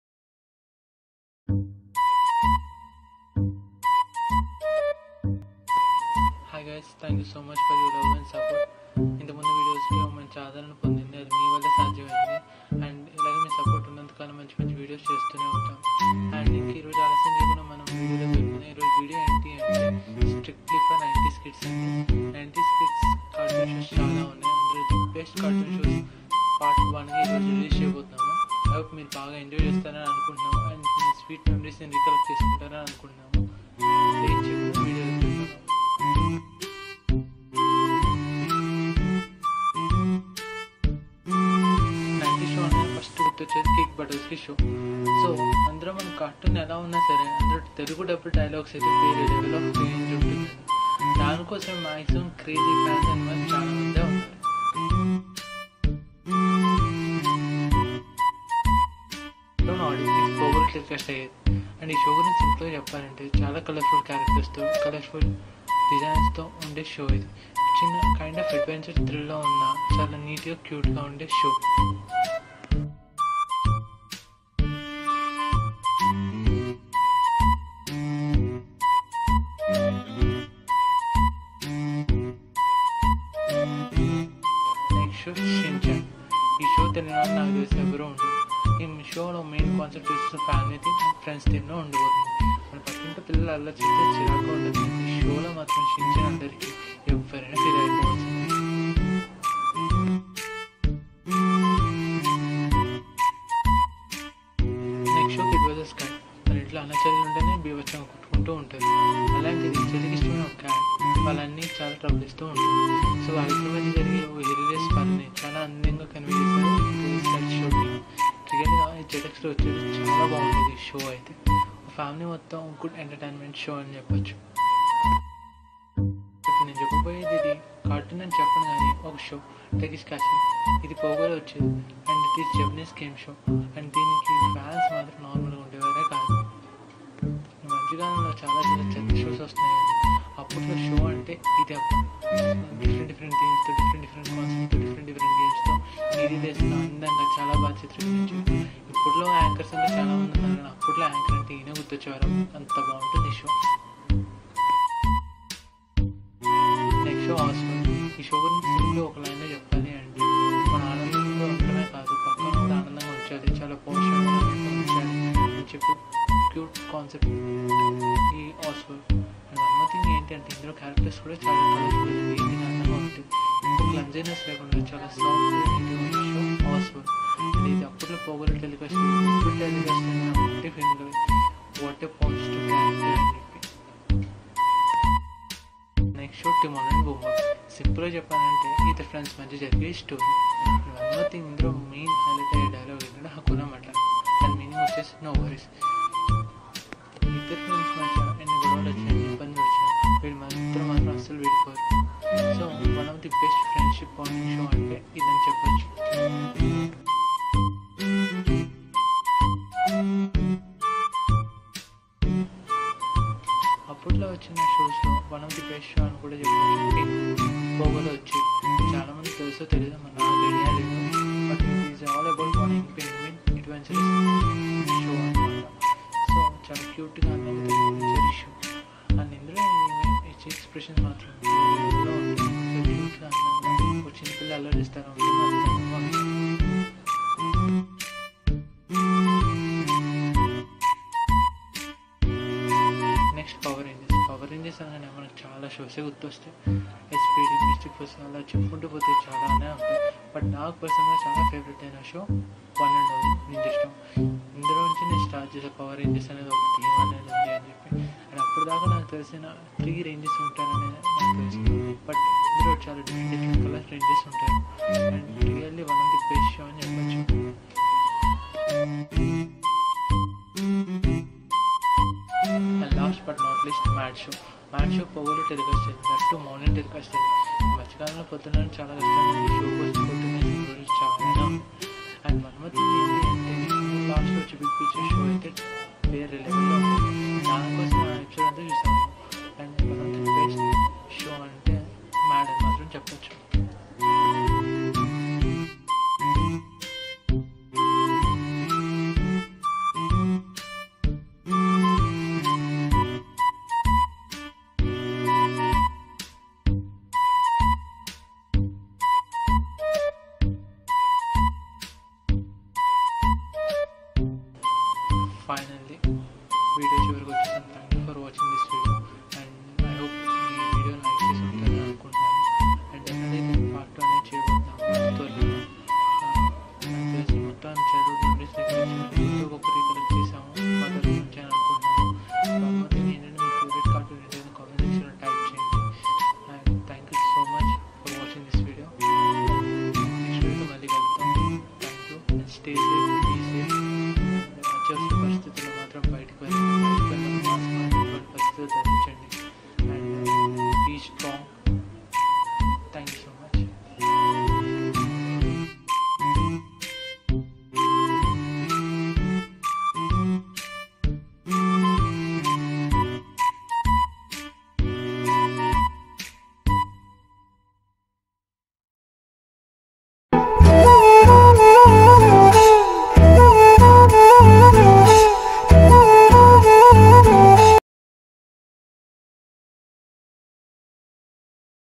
Top 10 Most 90s TV Shows Of All Time | Cartoon Shows | 90’s Nostalgia
In this 90's  TV Cartoons Video I am gonna show you guys some of our childhood shows from Cartoon Network, Disney, Jetix, Pogo

The decade brought an explosion of creativity in the field of animation, and as a result, the '90s produced cartoon masterpieces that have stood the test of time. The cartoons you watched as a kid not only hold up today, but in some cases they're even better as seen through your adult eyes